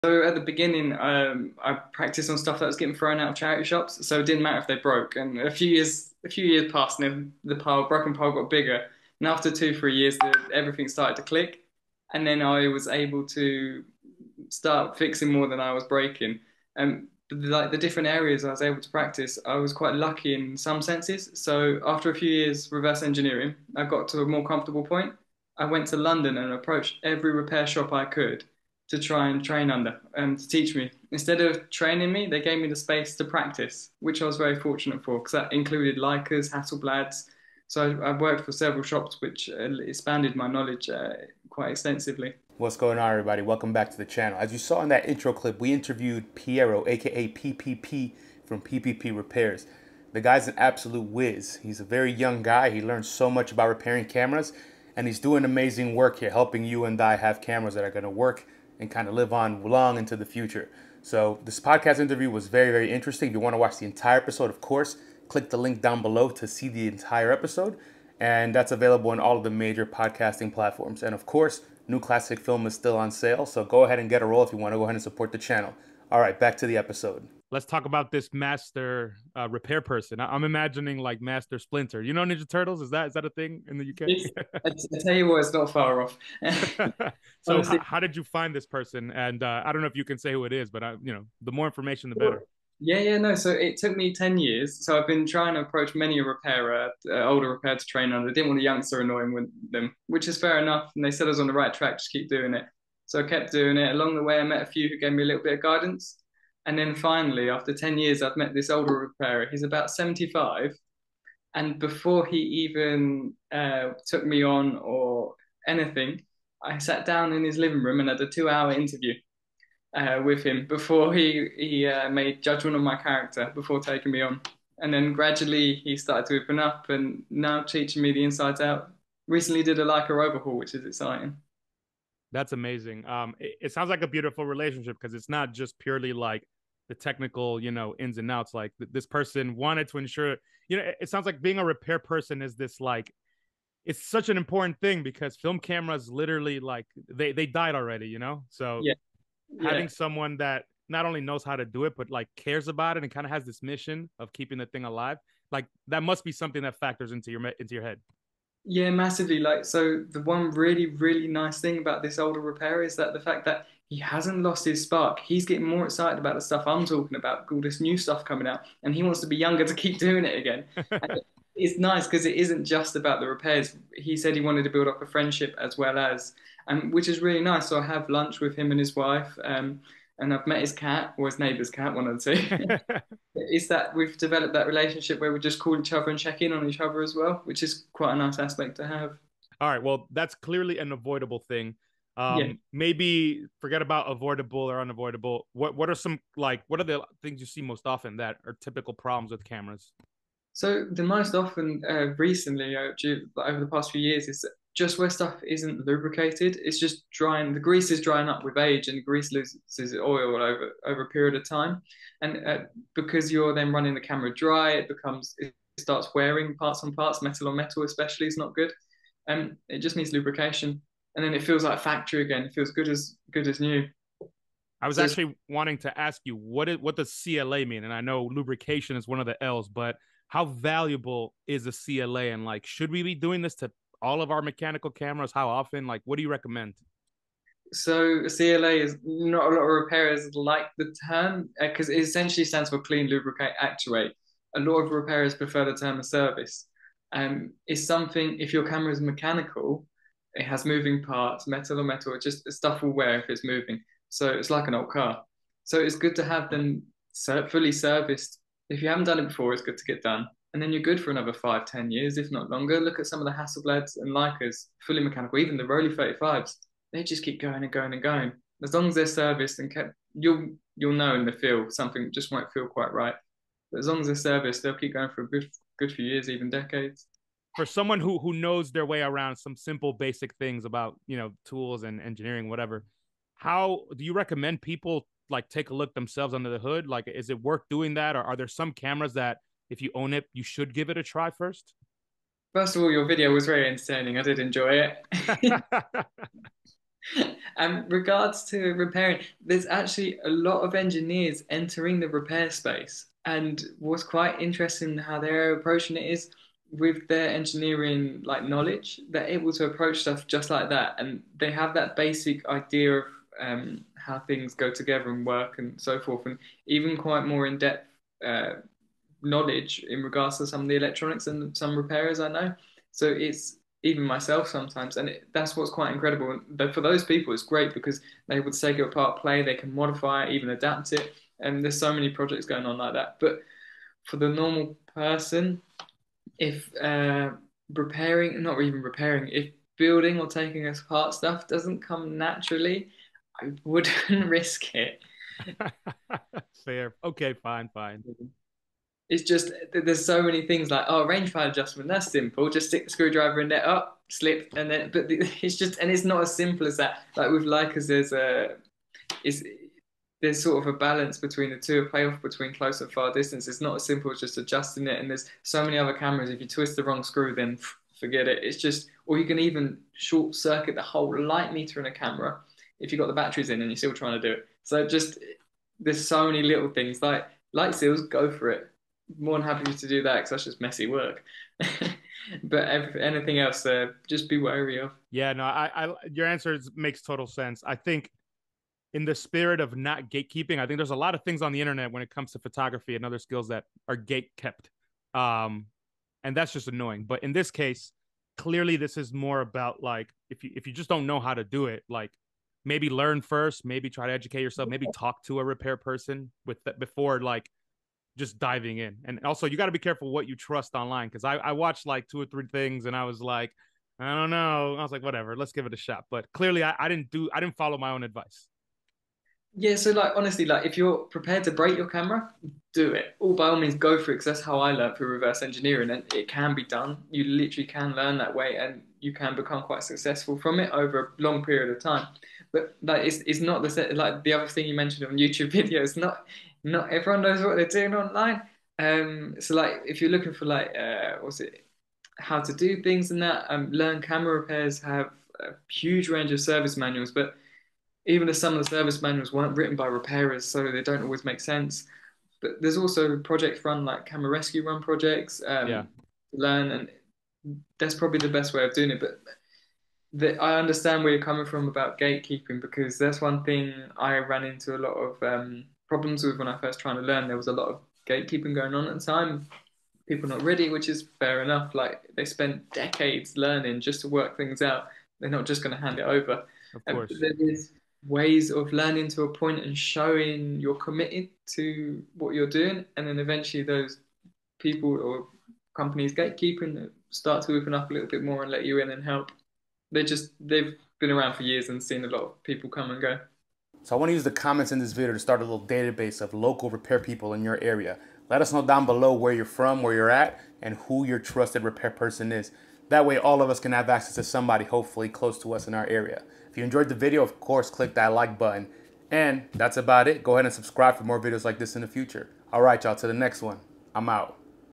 keeping film cameras alive ... who is "PPP" and how did he learn so much at such a young age?
I spoke with Piero PPP about his camera repair journey and how he got all of the knowledge that he has. He can repair basically any film camera and has been doing so with much satisfaction.

0:00 how PPP got started
1:17 the camera repair sensei
3:09 the most typical camera issues
3:44 why is CLA a bad term
4:37 should you DIY repair?
6:24 laughing at my repair mistakes